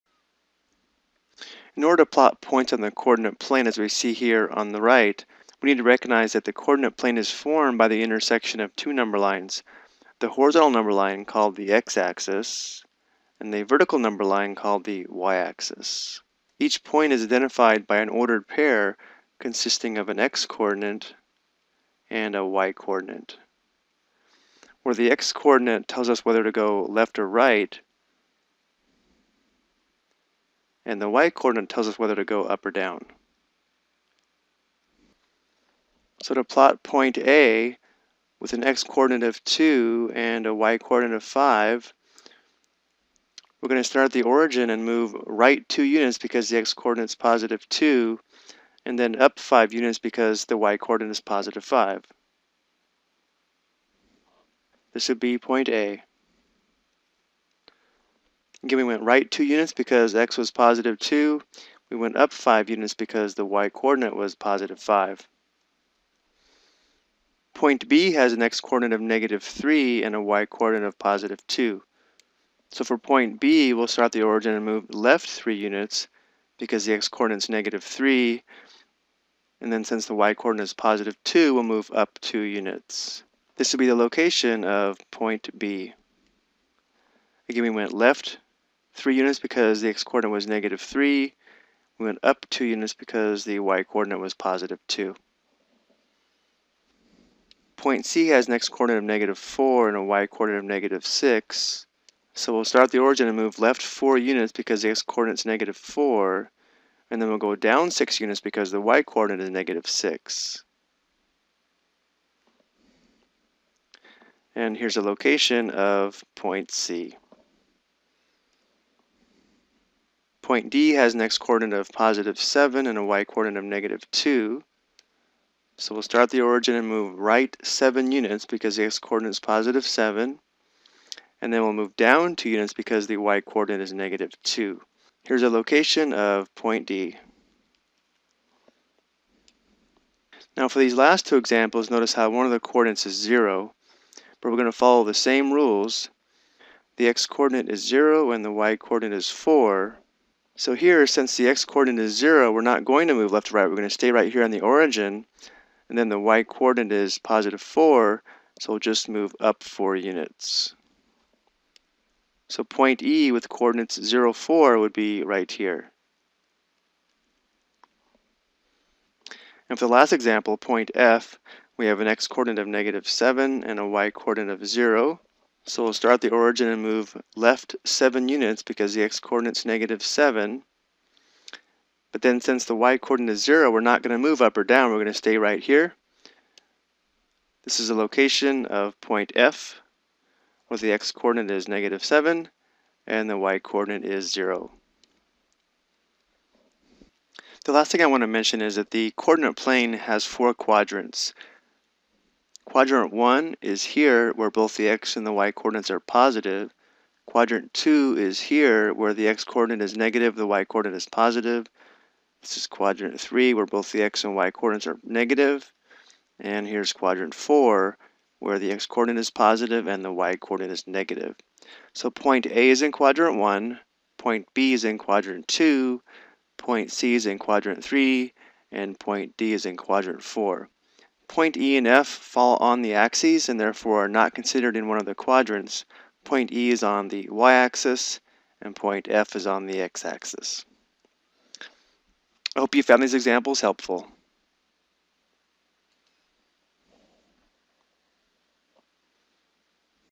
Ex: Plotting Points on the Coordinate Plane
This video provides examples of how to plot points on the coordinate plane.